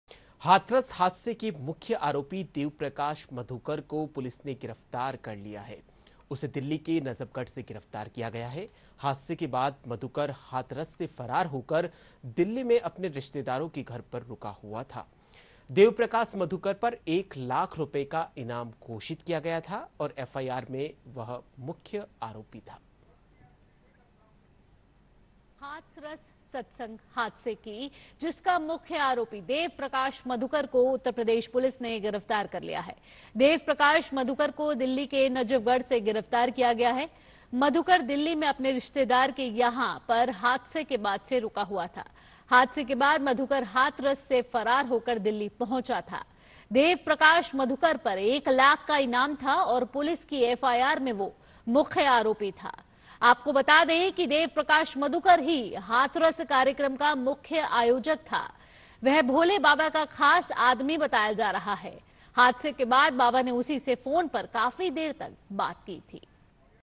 हाथरस हादसे का मुख्य आरोपी मधुकर गिरफ्तार, एक लाख का इनामी था बाबा साकार विश्व हरि का सेवादार
उत्तर प्रदेश के हाथरस में भगदड़ के मुख्य आरोपी देव प्रकाश मधुकर को गिरफ्तार कर लिया गया है। उस पर एक लाख का इनाम था। बाबा साकार हरि विश्व के वकील एपी सिंह ने बताया कि हाथरस सत्संग कांड के मुख्य आरोपी वेद प्रकाश मधुकर को पुलिस को सौंप दिया गया है। दिल्ली में उसे एसटीएफ और एसआईटी की टीम को 10 बजे सौंपा गया।